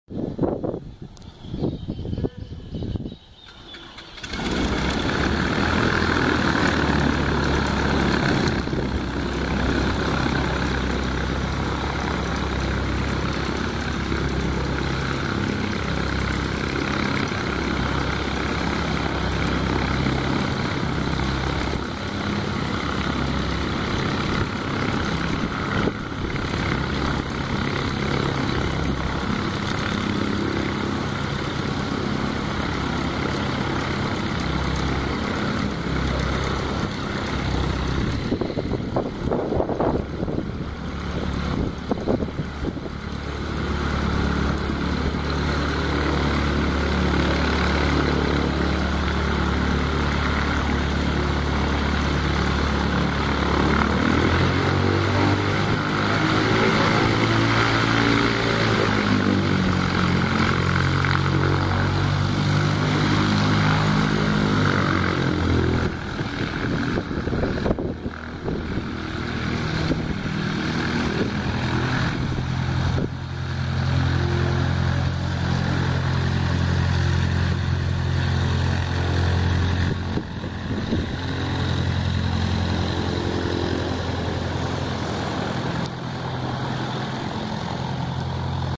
Cessna 150 CX-SAS Start Up Ángel Adami
Encendido del CX-SAS de SAS escuela de vuelo.